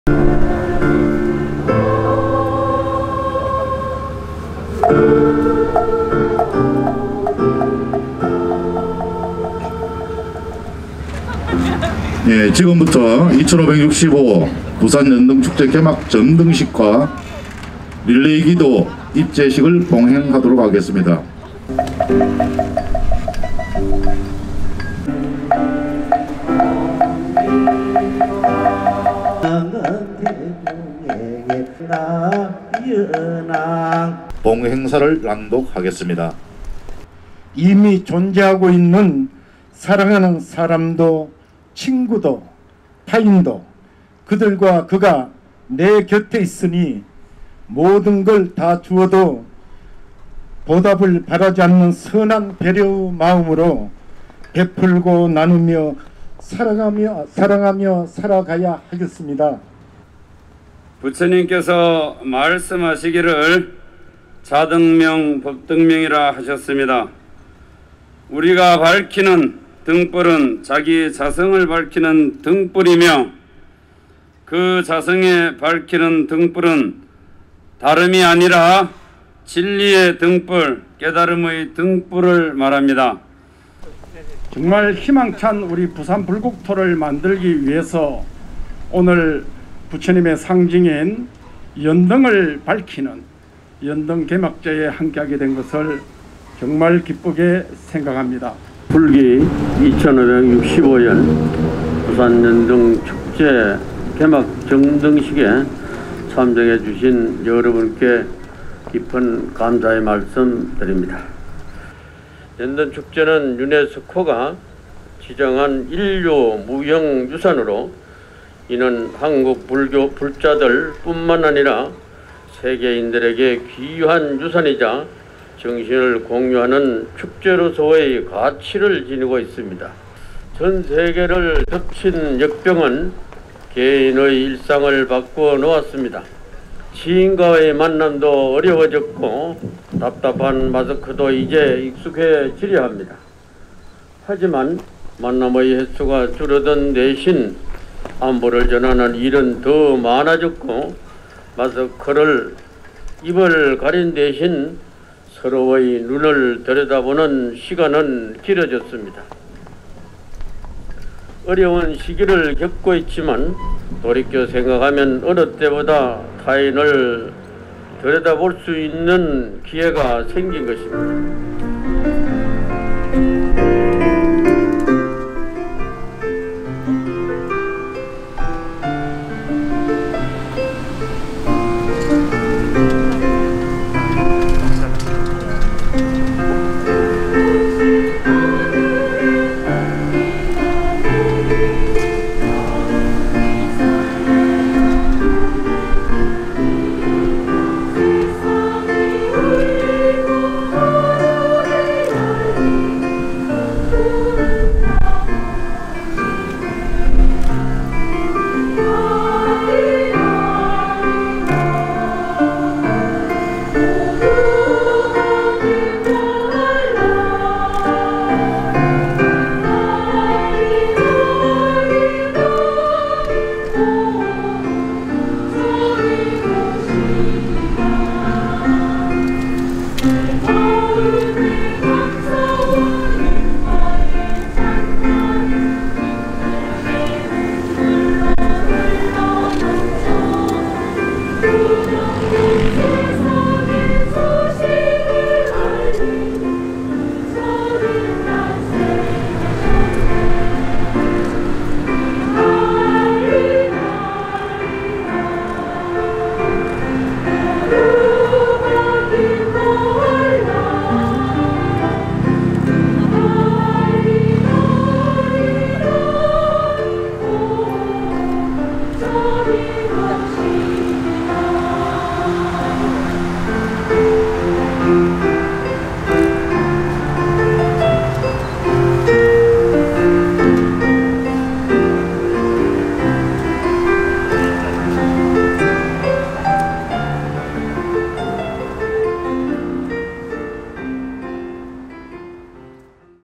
희망과 치유를 위한 ‘2021 부산연등축제’ 개막
부산연등축제조직위원회(위원장 경선스님, 금정총림 범어사 주지)는 5월1일 송상현광장 특설무대에서 ‘불기2565년 부처님오신날 부산연등축제 개막 점등식’을 봉행했습니다.

작년에 이어 코로나19로 인해 봉축연합대회와 연등행렬이 취소된 부산연등축제는 이날 점등식을 시작으로 5월16일까지 16일간 송상현 광장에서 장엄등을 밝히며 희망과 치유를 기원하는 릴레이 기도로 진행됩니다.